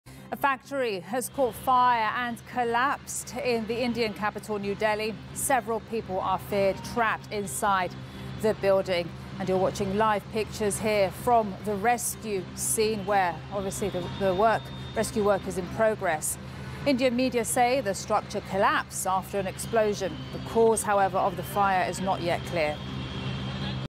Live pictures from the factory fire in New Delhi, India
A factory has caught fire and collapsed in the Indian capital New Delhi. Several people are feared trapped inside the building. You are watching LIVE pictures from the scene, where a rescue operation is under way. Indian media say the structure collapsed after an explosion. The cause of fire is not clear. Last month, a fire believed to be caused by an electrical short circuit engulfed a building in New Delhi, killing at least 43 people.